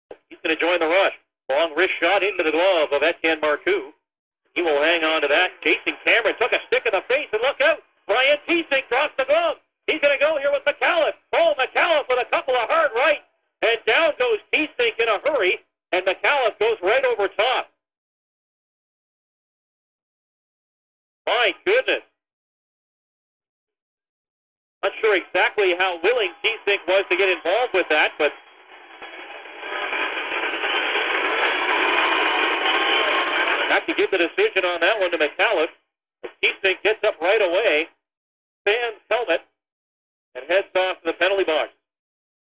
Alex Micallef vs Ryan Tesink Oct 10, 2010
Alex Micallef vs Ryan Tesink from the Montreal Junior at Saint John Sea Dogs game on Oct 10, 2010.